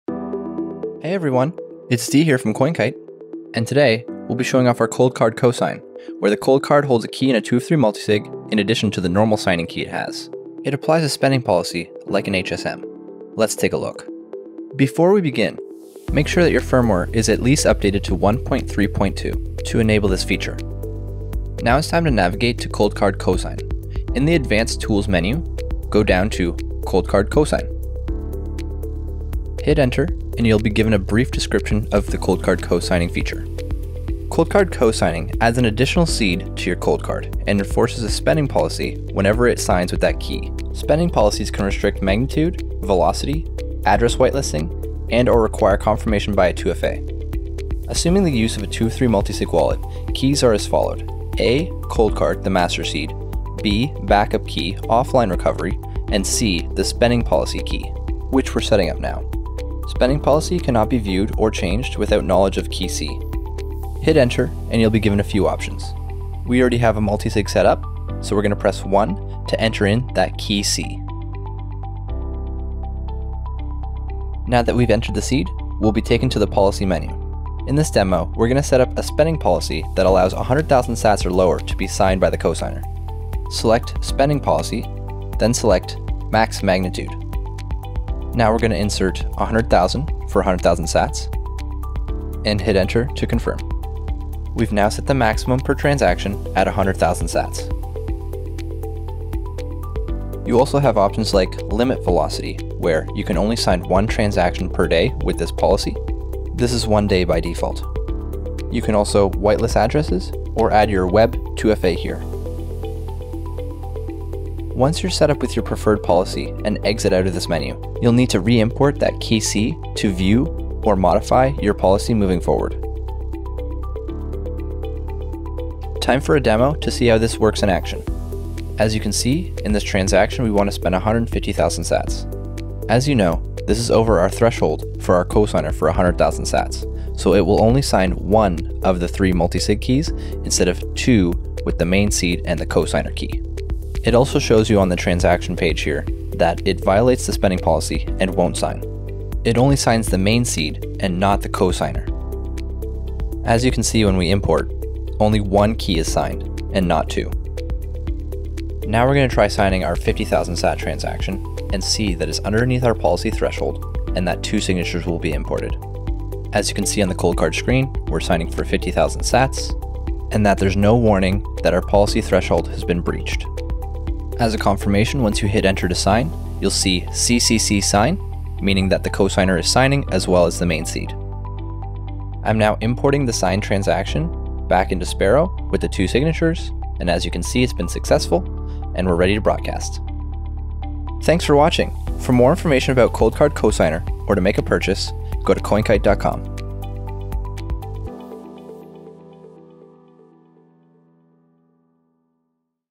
COLDCARD Co-Sign (CCC)
In this tutorial, we demonstrate how to use the COLDCARD Co-Sign function on the Coldcard Q.